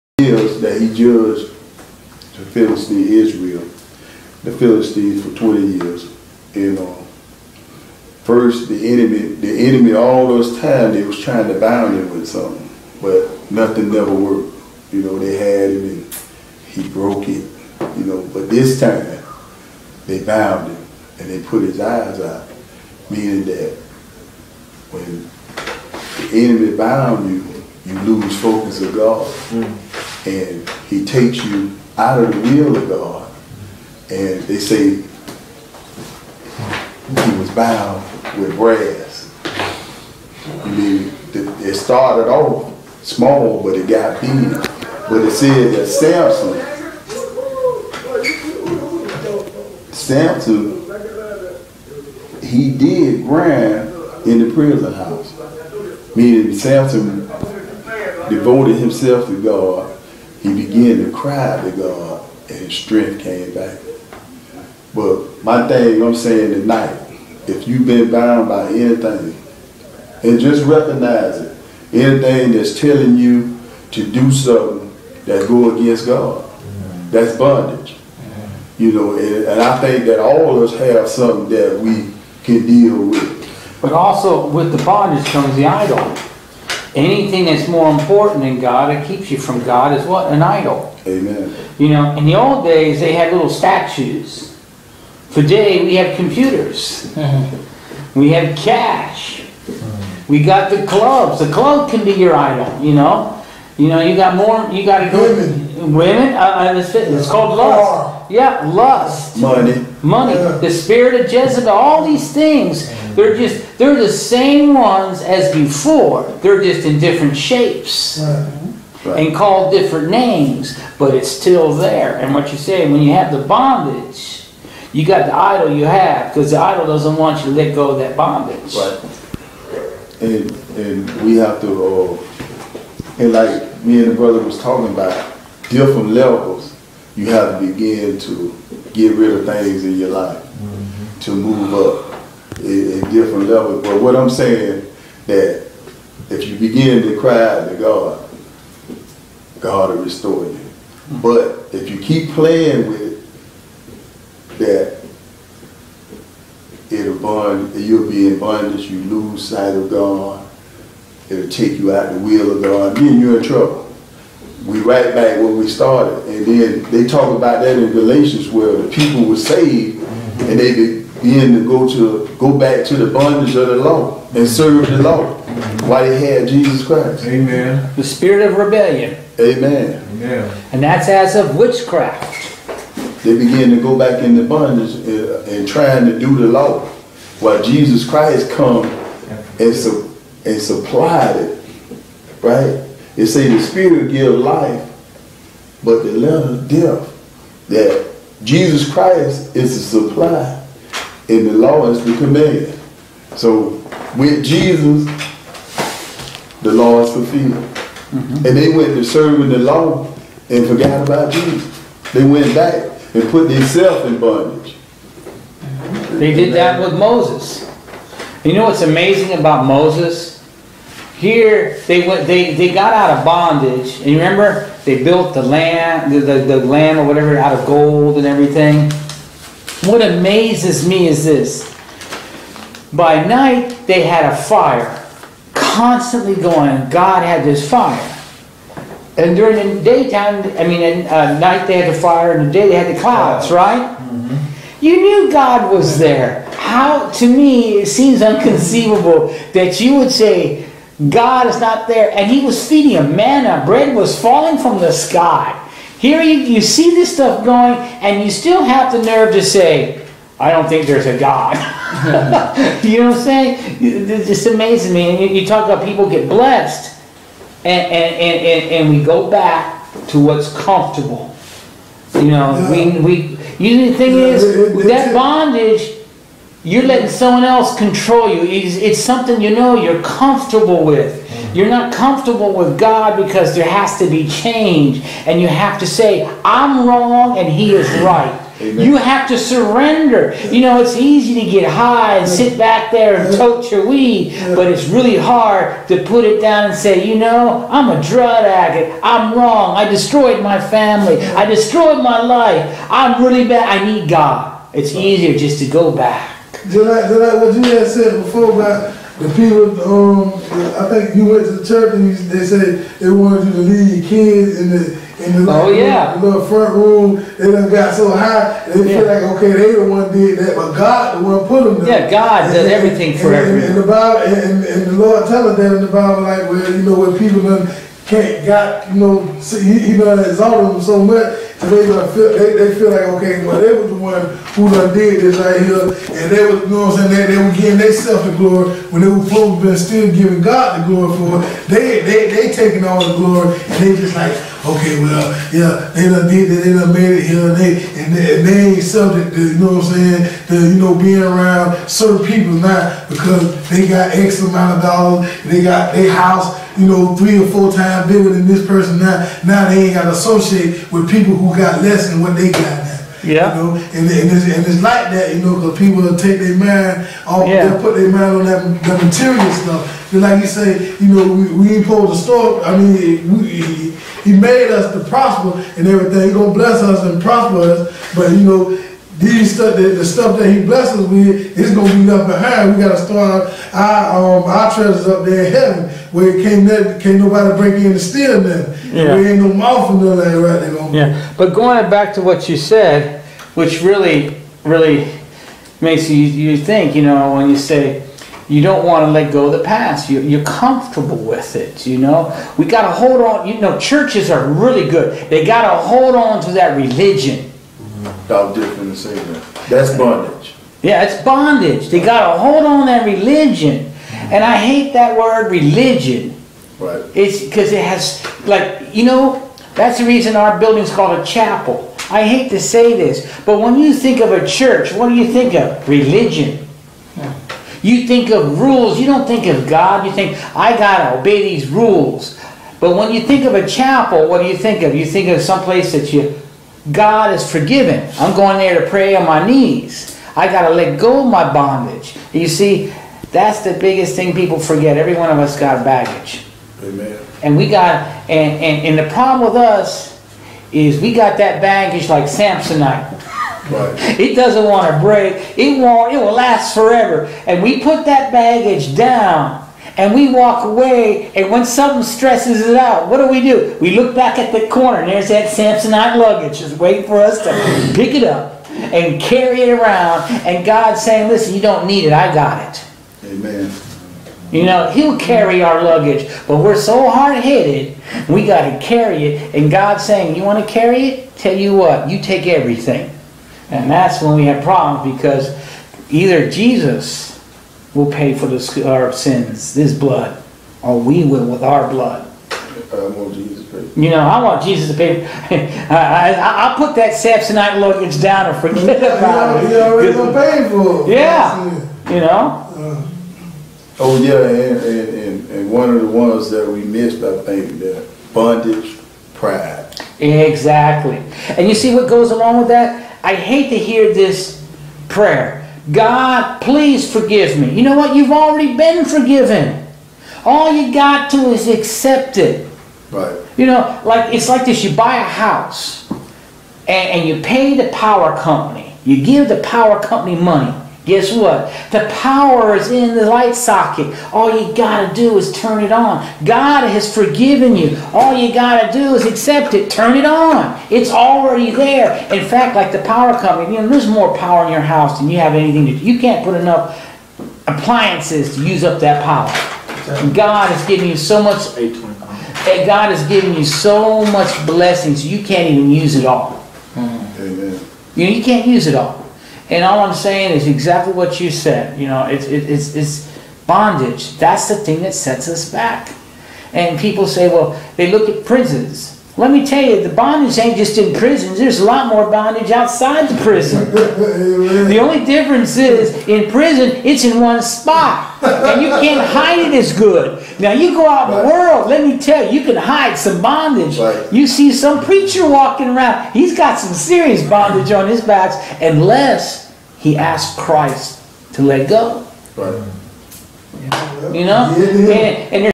( Part 4 ) Join Our Prison Inmate Bible Study with Church Of God Chaplain Ian Perry 12-13-2011
This is a Live recorded Prison Inmate Bible Study Weekly Bible study at a locked down facility. The Inmates here are Locked up in Florida. This Study is on the book of Acts chapter 12. there will be five parts. These Inmates want to share the Gospel Of Jesus With you.